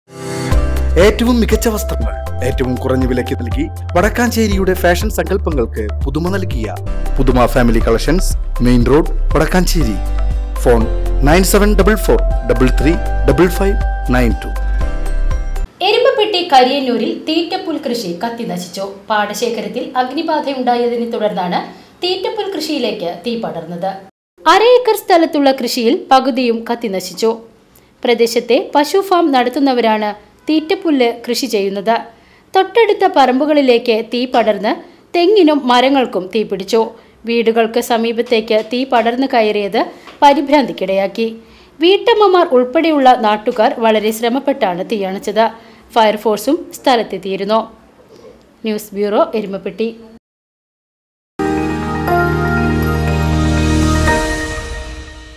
എരുമപ്പെട്ടി: കരിയന്നൂരിൽ തീറ്റപ്പുൽ കൃഷി കത്തിനശിച്ചു
എരുമപ്പെട്ടി: കരിയന്നൂരിൽ തീറ്റപ്പുൽ കൃഷി കത്തിനശിച്ചു; പാടശേഖരത്തിലുണ്ടായ അഗ്നിബാധയെ തുടർന്നാണ് തീറ്റപ്പുൽ കൃഷിയിലേക്ക് തീ പടർന്നത്

VCV NEWS

VCV NEWS CHANNEL
Wadakkanchery, Thrissur